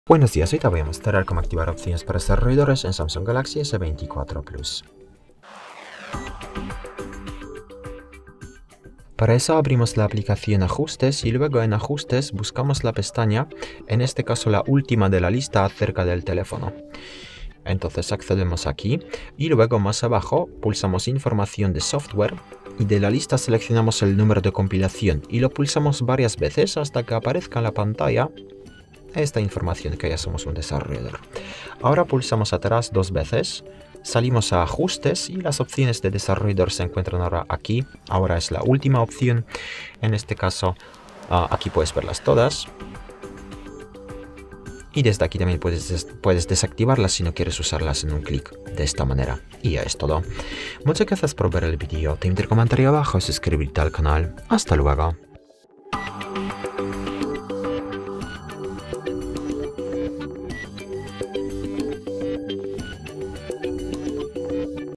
Cómo activar las opciones para desarrolladores en mi Samsung Galaxy S24+
¿Sabías que tu Samsung Galaxy S24+ tiene un conjunto de opciones avanzadas conocidas como "Opciones para desarrolladores"? En este tutorial, te mostramos cómo acceder a estas opciones ocultas y cómo utilizarlas para personalizar y optimizar tu dispositivo. Descubre cómo activar funciones experimentales, mejorar el rendimiento y realizar ajustes avanzados en tu Galaxy S24+.

¿Cuáles son las funciones disponibles en las opciones para desarrolladores de un Galaxy S24 Plus?
¿Por qué debería habilitar las opciones para desarrolladores en mi Samsung S24+?
¿Es seguro hacer ajustes en las opciones para desarrolladores de un Samsung Galaxy S24+?